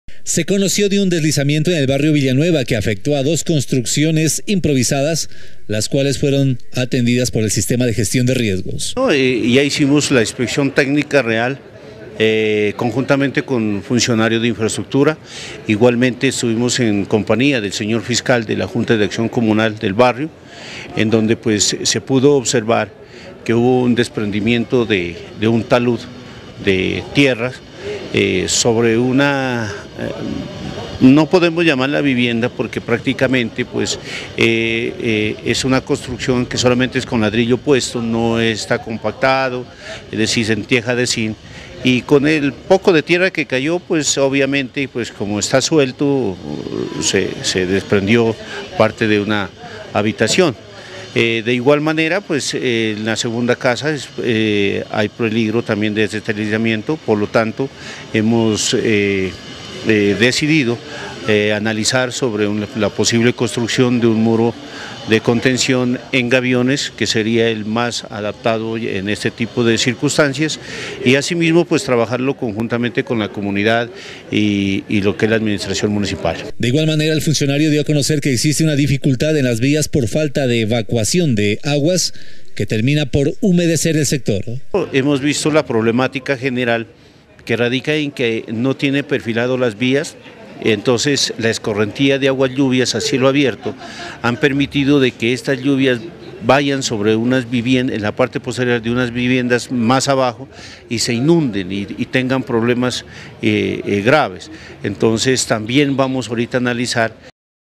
CNC Noticias Pasto -Barrio Villanueva afectado por lluvia (Ipiales)
Un barrio de Ipiales fue afectado por las fuertes lluvias, las autoridades entregaron las primeras ayudas y esperan medidas especiales para evitar mayores afectaciones